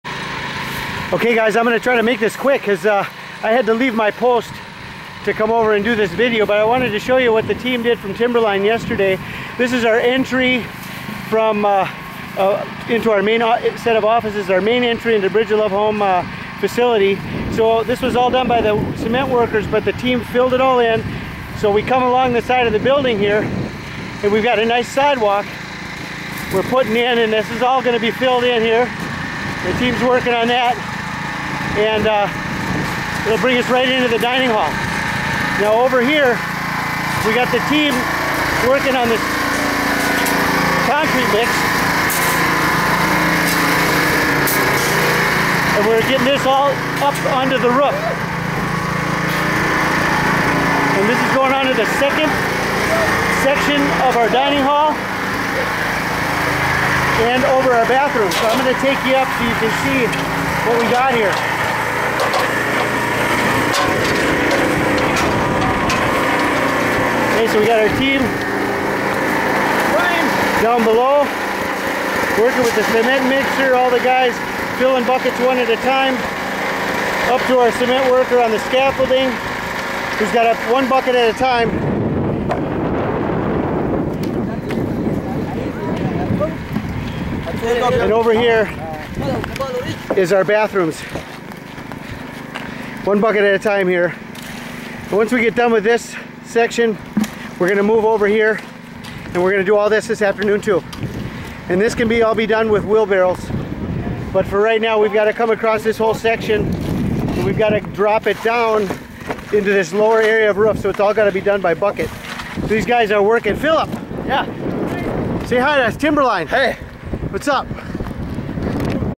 Timberline Team 1, 2012, days 1 and 2
Front entry to facility, roof pour on dining hall and bathrooms.